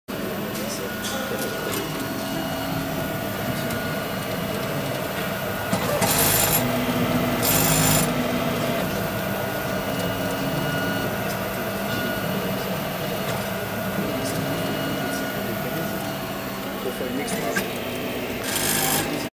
Reprap asimov
Printing a plastic piece to hold infrared sensor to make a linear encoder. The piece was first design with kokopelli, then with openscad.
The reprap is asimov one from plateformeC